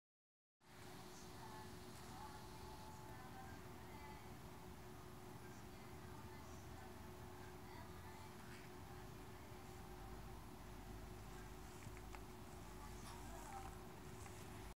Dog vs Cat Toy
The cat lets the dog take his toy for a spin.